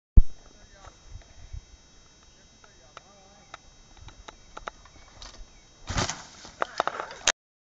drop 4,50mt costaema
drop di 4,50mt con atterraggio in piano!!